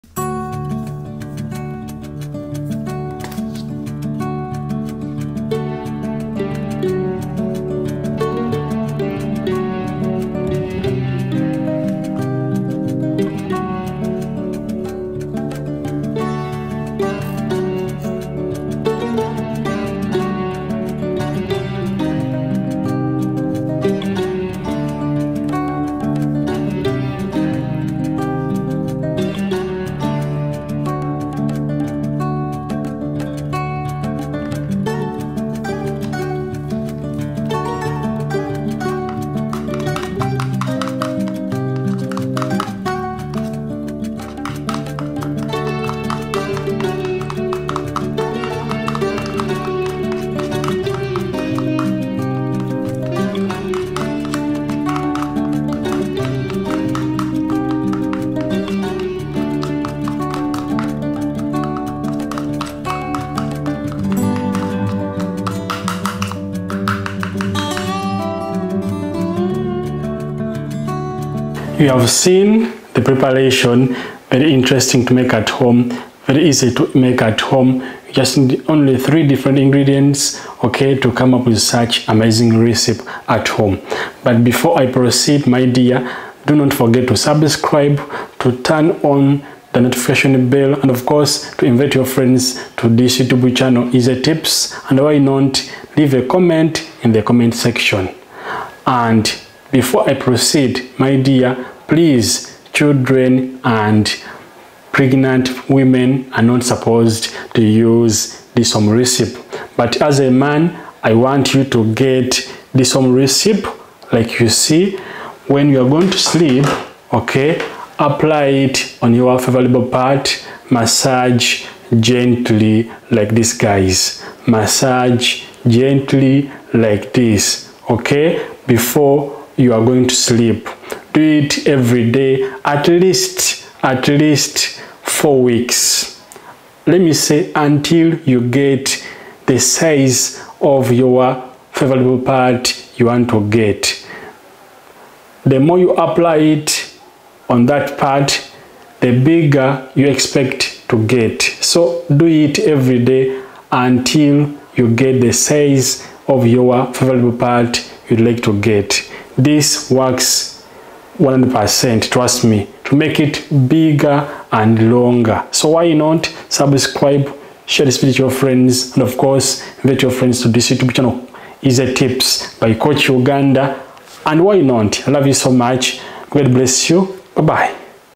Toothpaste & Vaseline Routine – Tried, Reviewed & Honest Result
You’ve probably seen viral videos claiming that mixing Vaseline and Colgate can do amazing things — but is it true? In this video, we put this popular home hack to the test and explain the real science behind it.

You’ll learn:
✅ What happens when petroleum jelly (Vaseline) and toothpaste (Colgate) are combined
✅ The truth about common uses you might have seen online
✅ Whether it’s safe for your skin, teeth, or other purposes
✅ Important tips before trying this mixture at home

We’ll clear up the myths, share what actually works, and help you avoid mistakes that could harm your skin or oral health.

💬 Have you ever tried mixing Vaseline and Colgate? Tell us your experience in the comments!